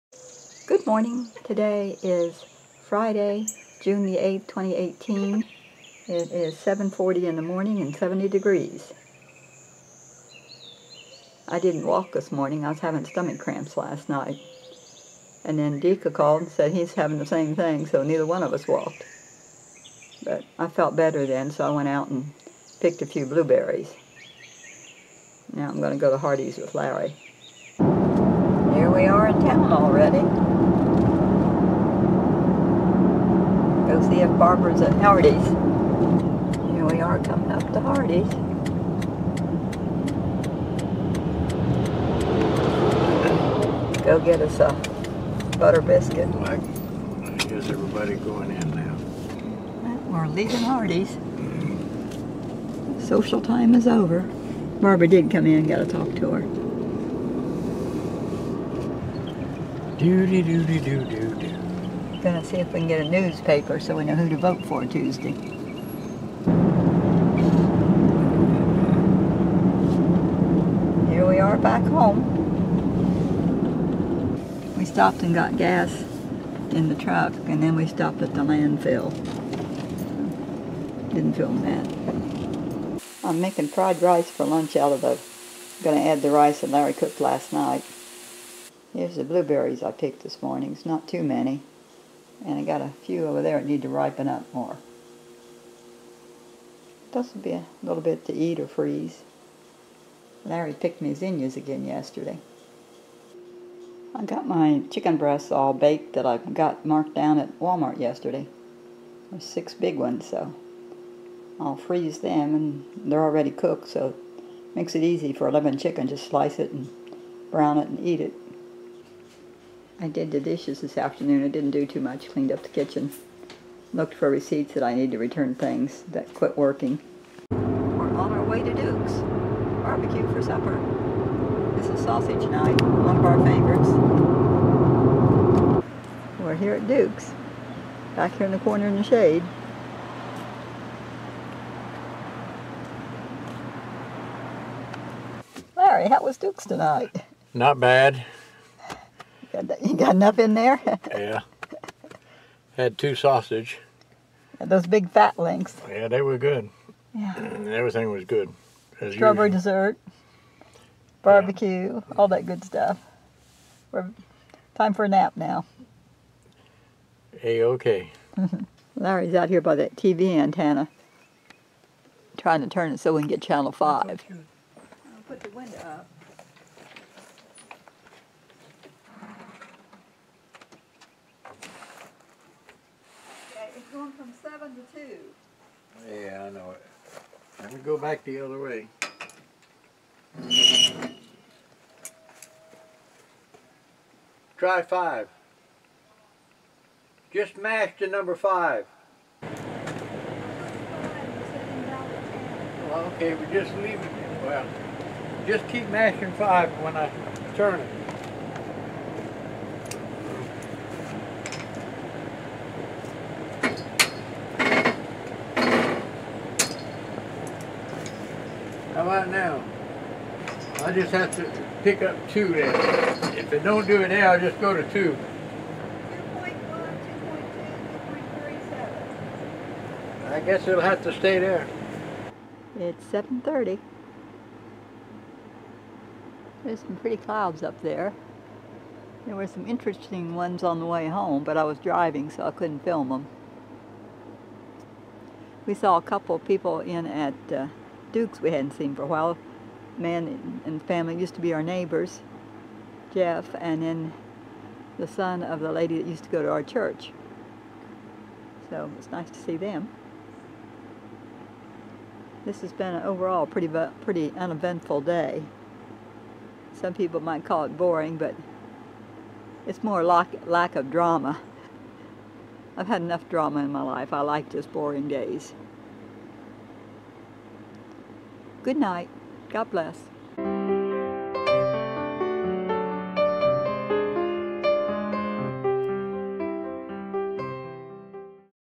20180608 Vlog - Turning the TV Antenna
I went to Hardee's with Larry.  We picked a few blueberries.  We got a newspaper at the grocery store.  We ate fried rice for lunch.  I cleaned the kitchen and froze chicken.  We went to Duke's BBQ for supper.